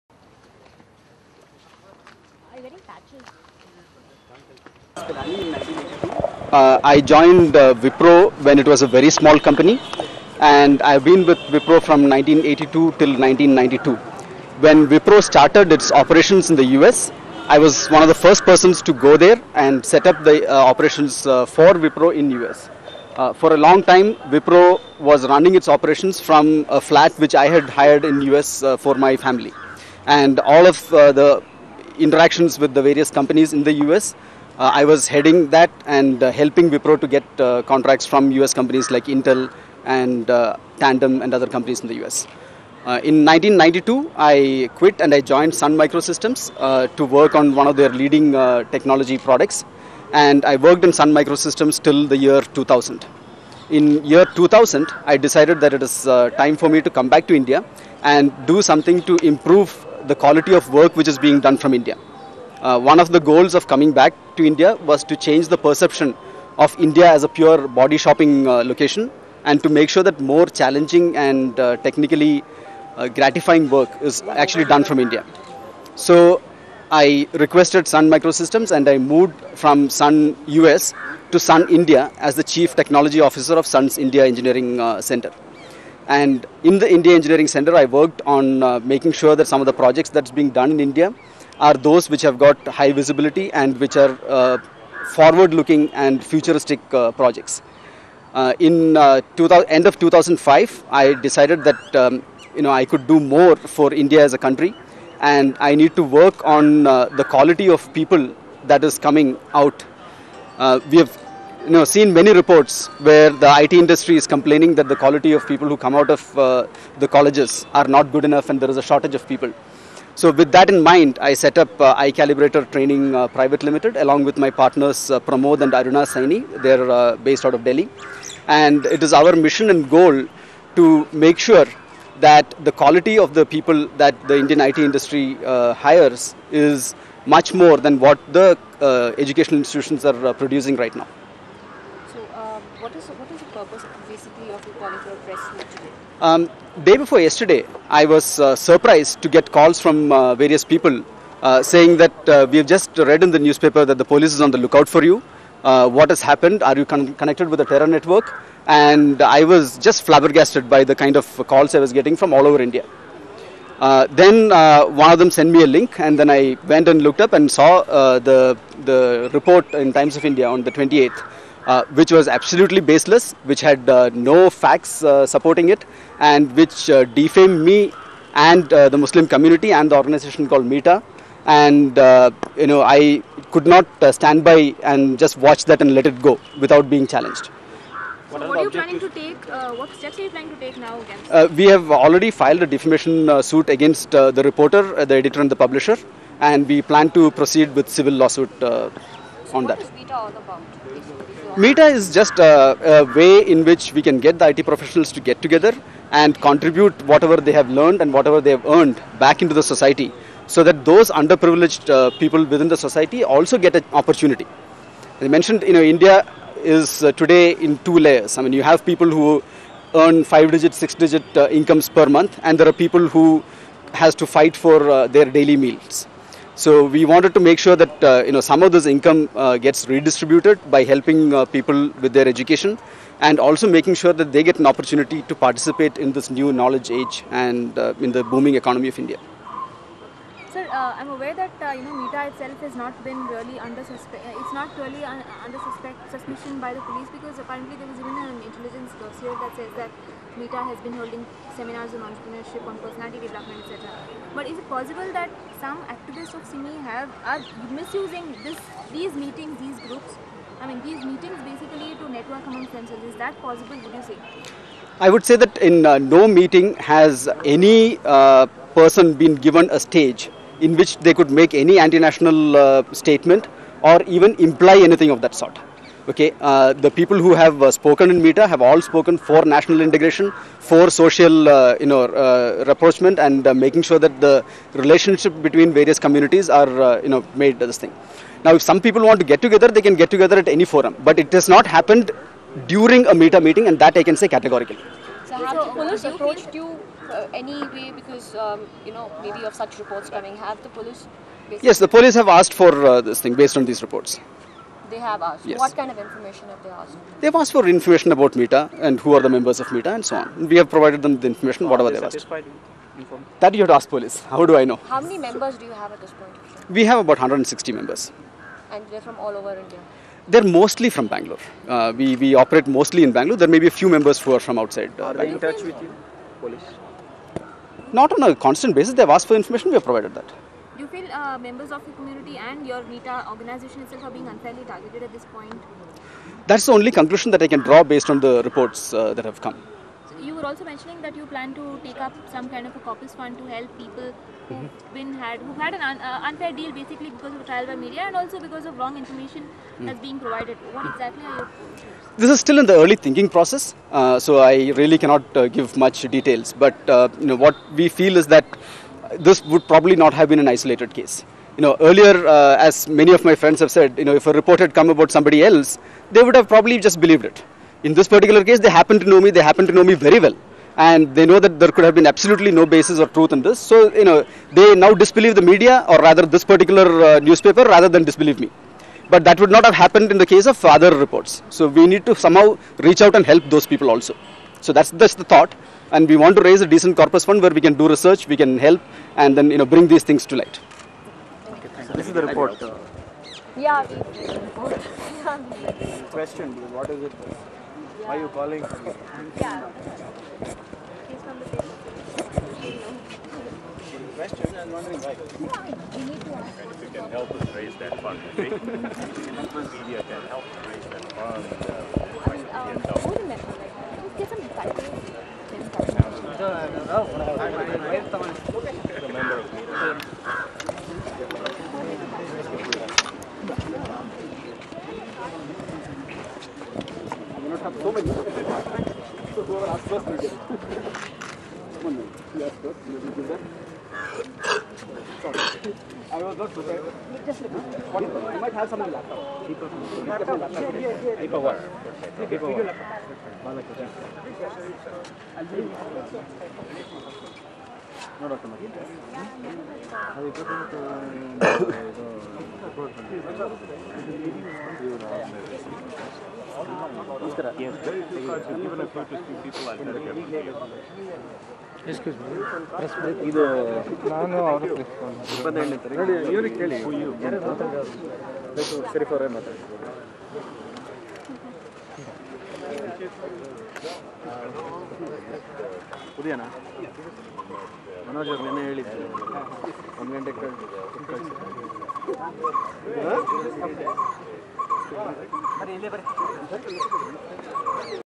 Part 4 - Press Conf - Defamation suit against Mr. Shiv Kumar
Mr. Sherif speaking to the media after a Press Conference on Sat 1 March refuting allegations of terrorist links against MITA.